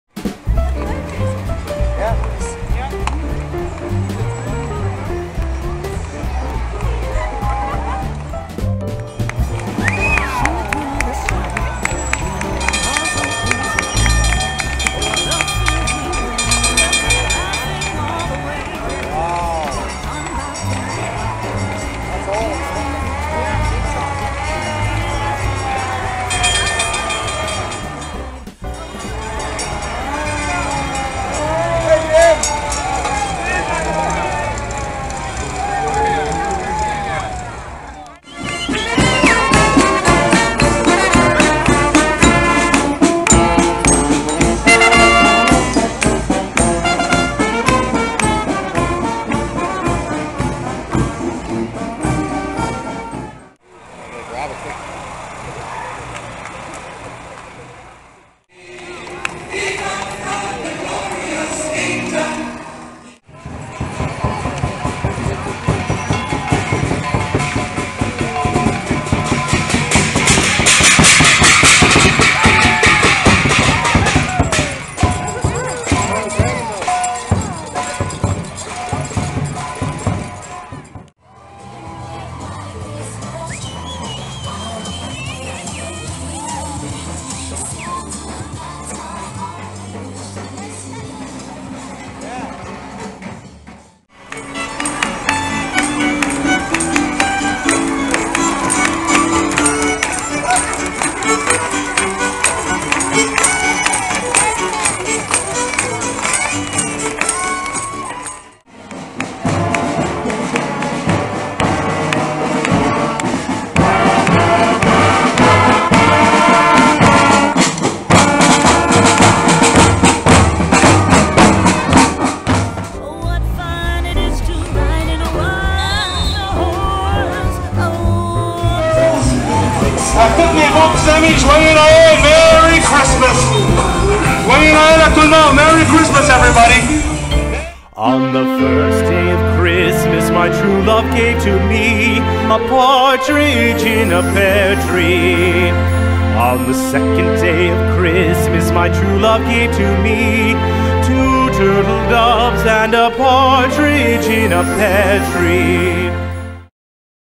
243 🎅 Wandering Around Ottawa ❄ Help Santa Toy Parade 🎄 2011
In 2011, Ottawa's Help Santa Toy Parade was held in the evening. The holiday lights adorning the floats and horses made this an extra special event, with a route that included the stretch of Bank Street that you see in thks video.